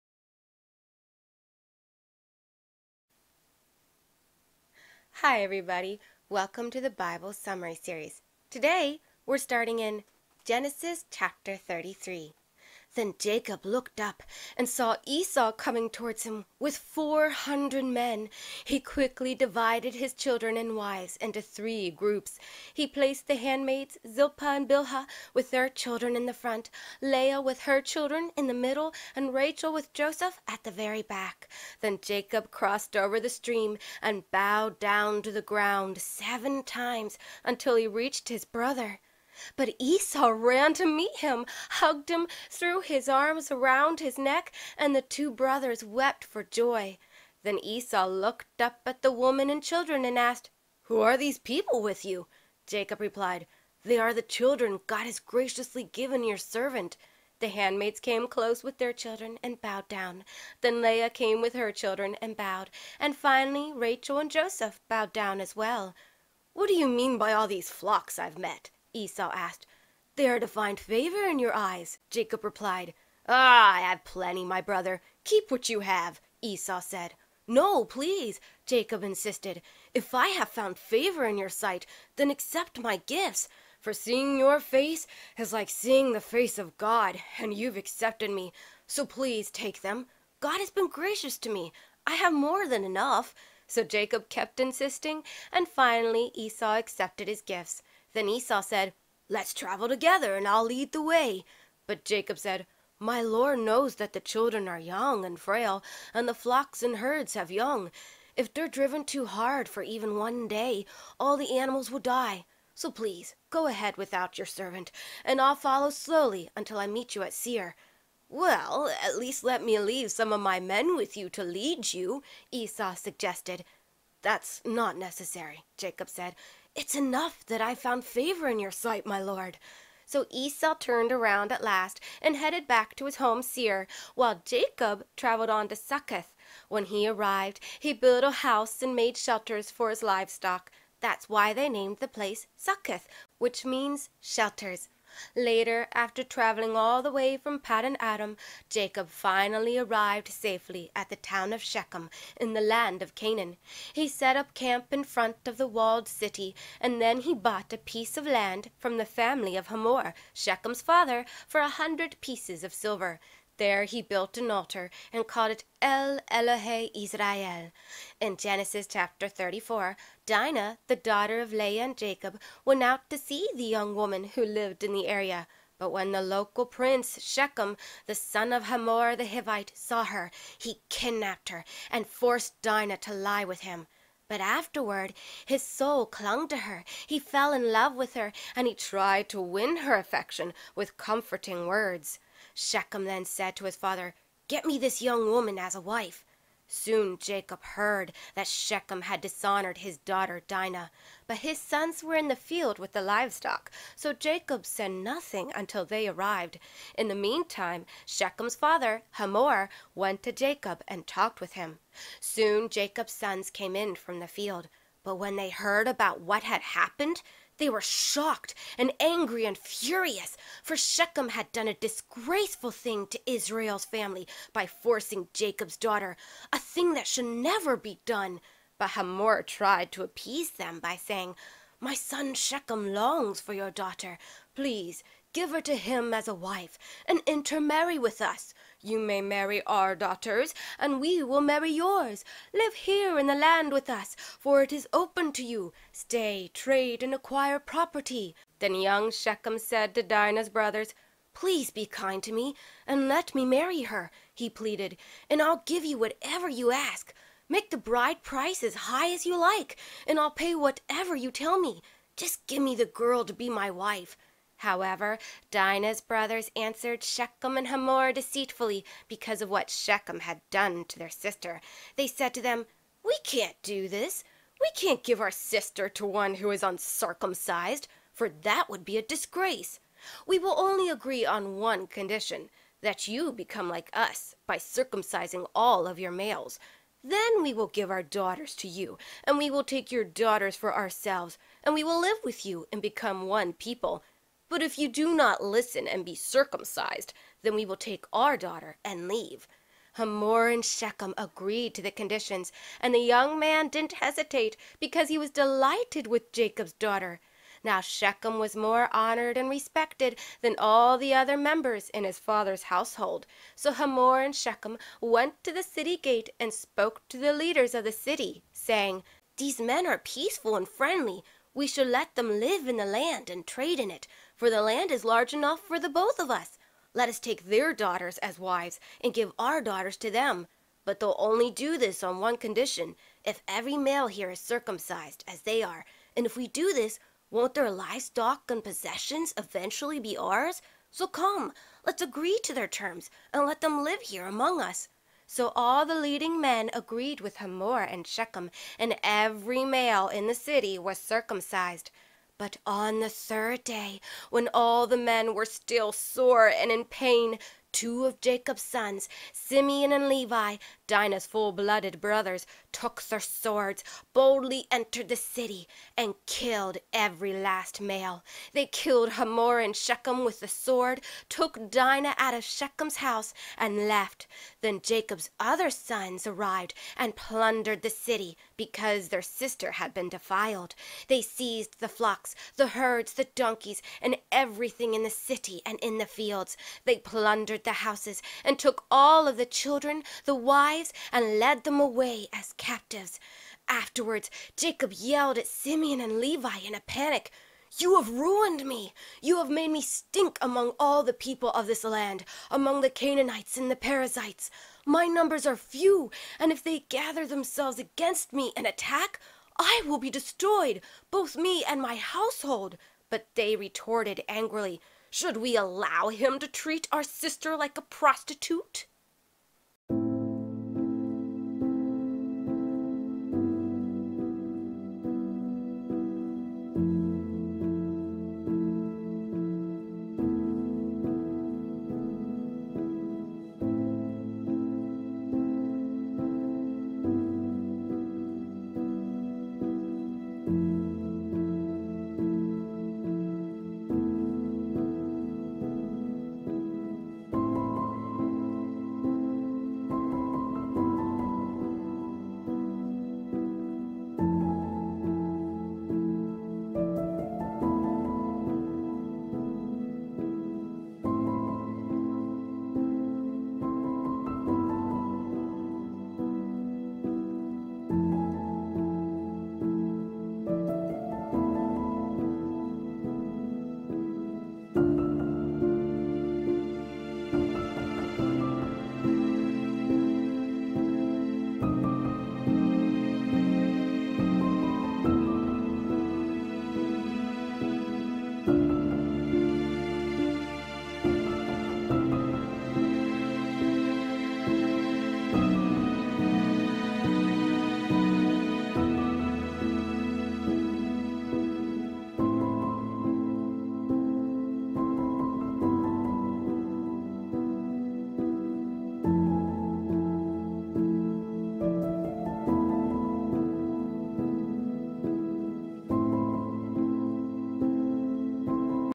Episode 11 - Jacob Reunites with Esau & the Defilement of Dinah - The Bible Summary Series
Genesis 33-34